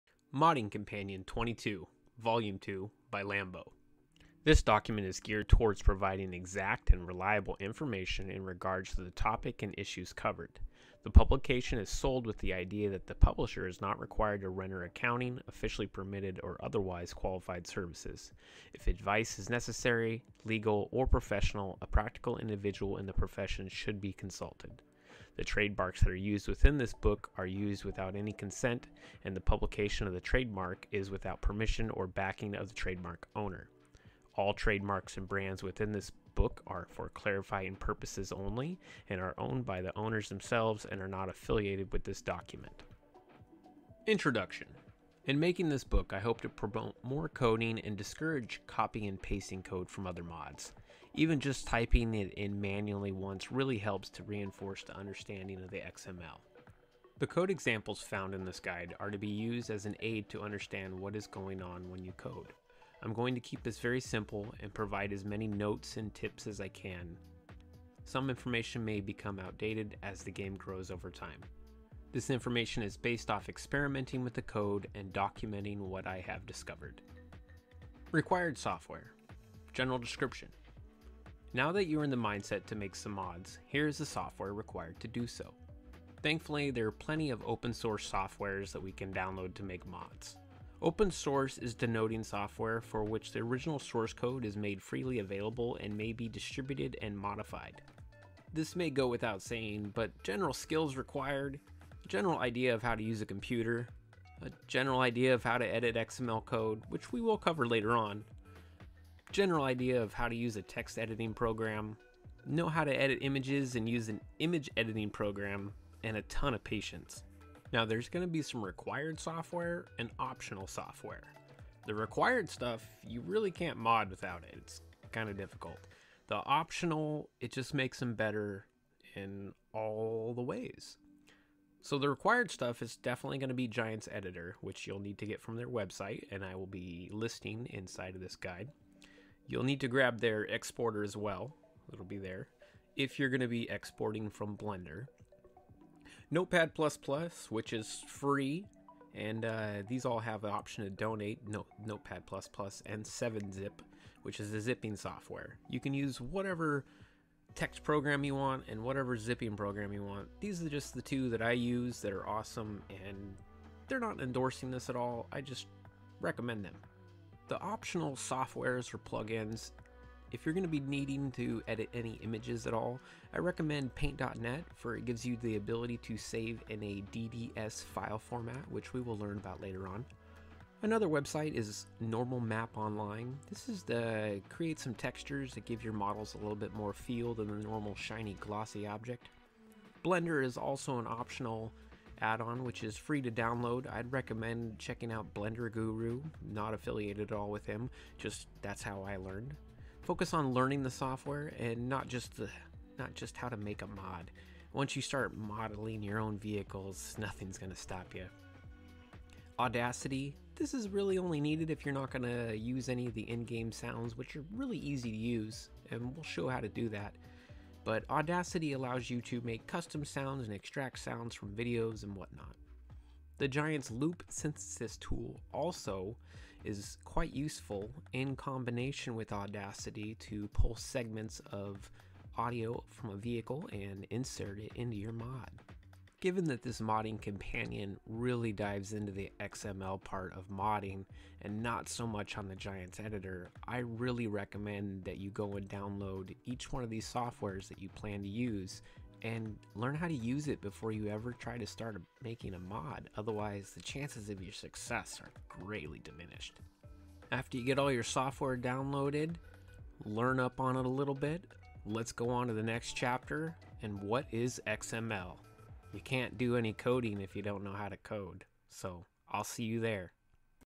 Modding Companion FS22 - Introduction to Modding Tools
Ep1
For Several years I have been trying to write a book, but each year the information gets updated in some aspect. But using the data I have gathered over the last half Decade I will once again try to compile all the XML Code I come across.
As this game evolves much of the information may become outdated.

TimeStamps:
00:03 - Disclaimer
00:43 - Introduction
01:17 - Required Software and skills
02:00 - Required Software
02:46 - Optional Software

OH and a NEW Discord Support team to help with issues!